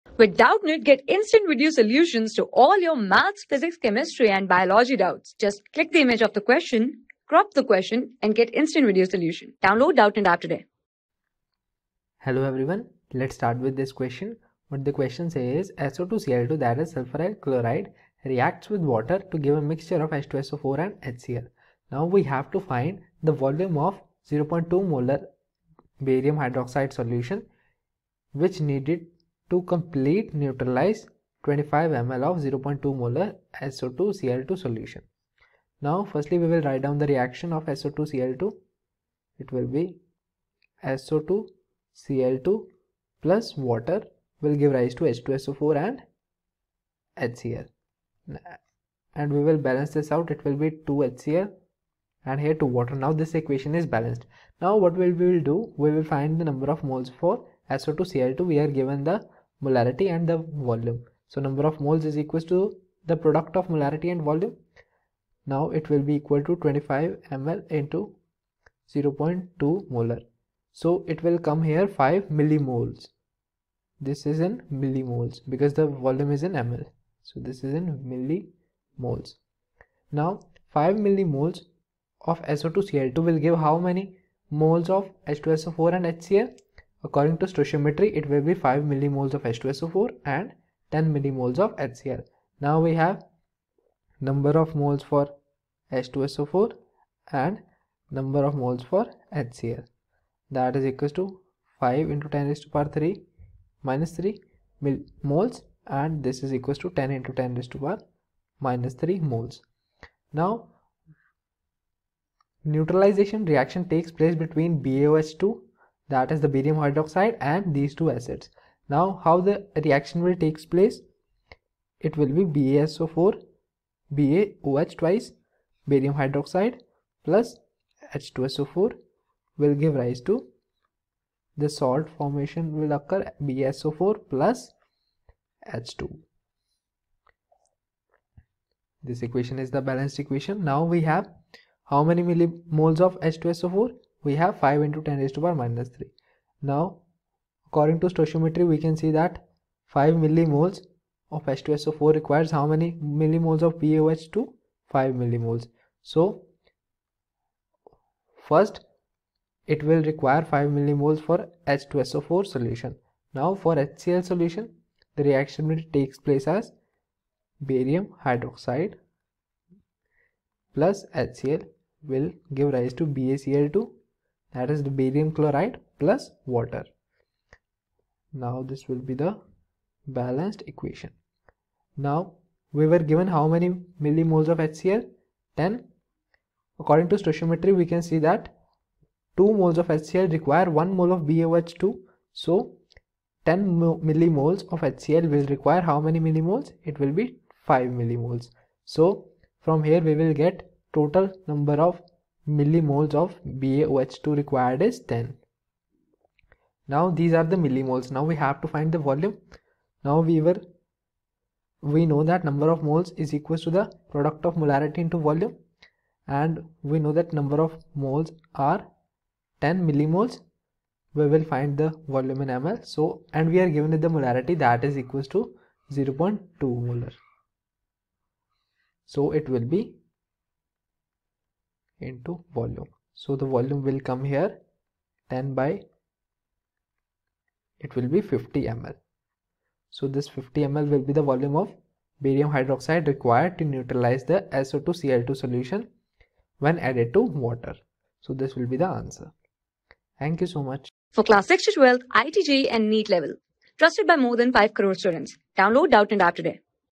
SO_2 CI_2 (sulphuryl chloride) reacts with water to give a mixture of H_2SO_4 and HCI. What volu...
SO_2 CI_2 (sulphuryl chloride) reacts with water to give a mixture of H_2SO_4 and HCI. What volume of 0.2 M Ba(OH)_2 in needed to completely neutralize 25mL of 0.2 MSO_2 CI_2 solution :
Class: 11
Subject: CHEMISTRY
Chapter: STOICHIOMETRY
Board:IIT JEE

👉 Have Any Query? Ask Us.